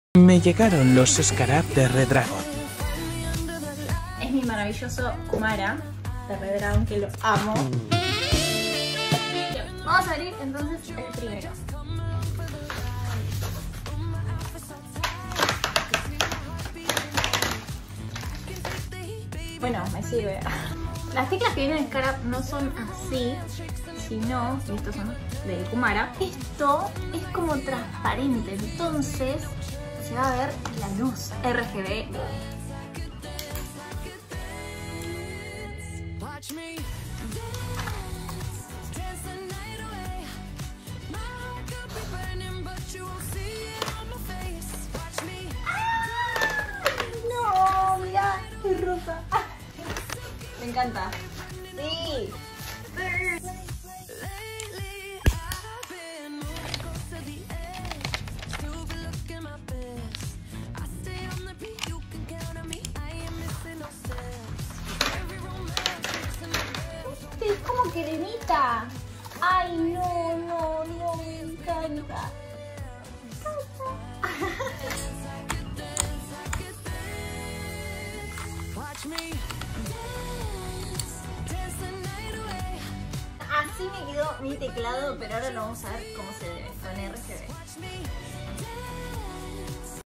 Redragon | Keycaps Scarabs 😍
@rocioregue nos muestra cómo le quedó su Kumara White, combinado con las Scarabs 😍😍
Le damos un 10/10 ✨✨

Les gustaría personalizar sus teclados?